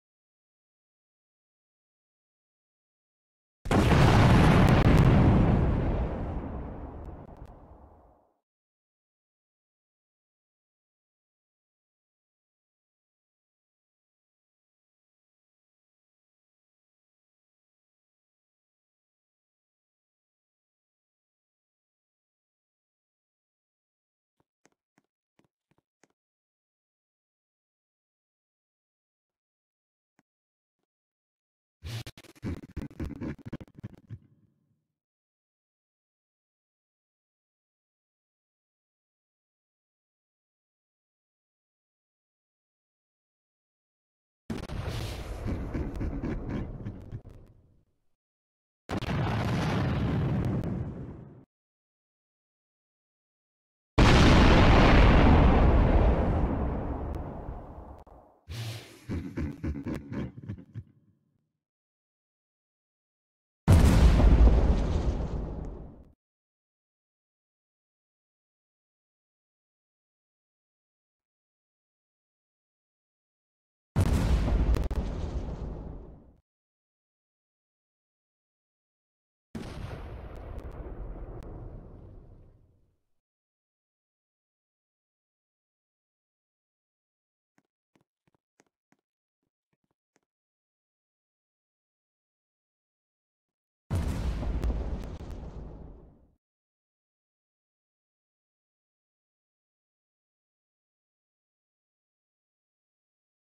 [TA] Voided Deity showcase
as you can see its obviously from void cup lmfao
void cup is from void chests which spawns in a rare chance